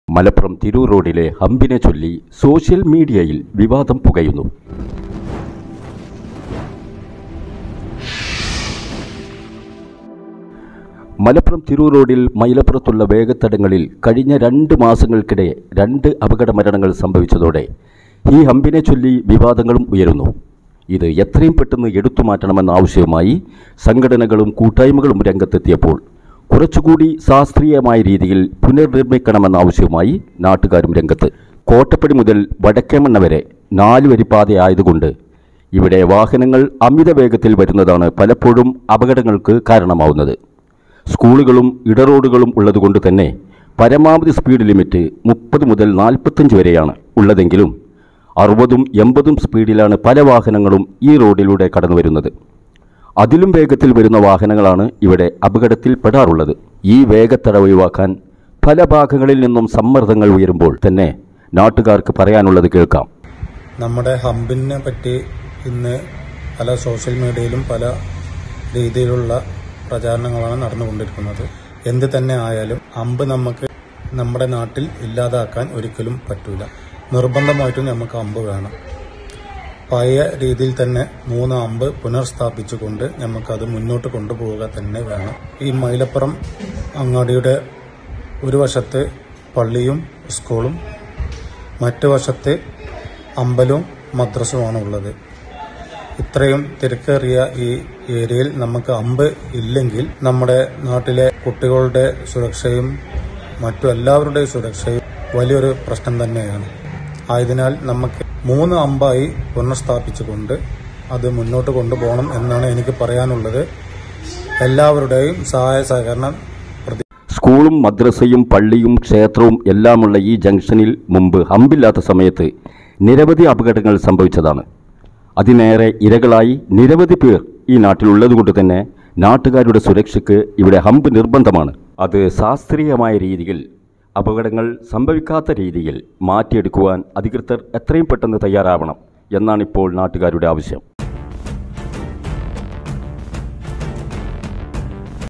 മൈലപ്പുറത്ത് ഹമ്പിനെചൊല്ലി വിവാദം, ഹമ്പ് മാറ്റണമെന്ന് യാത്രക്കാർ, ഇല്ലാതെ കഴിയില്ലെന്ന് നാട്ടുകാർ
മലപ്പുറം തിരൂർ റോഡിൽ മൈലപ്പുറത്തുള്ള വേഗത്തടകളിൽ കഴിഞ്ഞ രണ്ട് മാസങ്ങൾക്കിടെ രണ്ട് അപകട മരണങ്ങൾ സംഭവിച്ചതോടെ ഈ ഹമ്പിനെ ചൊല്ലി വിവാദങ്ങൾ ഉയരുന്നു. ഇത് എത്രയത്തെ പെട്ടെന്ന് എടുത്ത് മാറ്റണമെന്ന ആവശ്യവുമായി സംഘടനകളും കൂട്ടായ്മകളും രംഗത്തെത്തിയപ്പോൾ കുറച്ചു കൂടി ശാസ്ത്രീയമായ രീതിയിൽ പുനർ നിർമിക്കണമെന്ന ആവശ്യവുമായി നാട്ടുകാരും രംഗത്ത്.